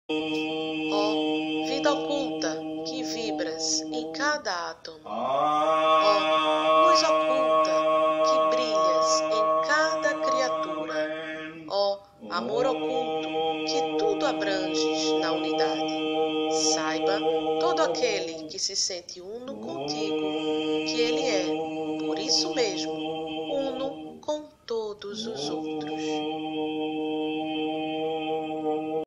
Mantra da Unidade
"Ó vida oculta que vibras em cada átomo,
Ó luz oculta que brilhas em cada criatura,
Ó amor oculto que tudo abranges na unidade,
Saiba todo aquele que se sente uno contigo,
Que ele é, por isso mesmo,uno com todos os outros."
Annie Besant

Om entoado por Paramahansa Yogananda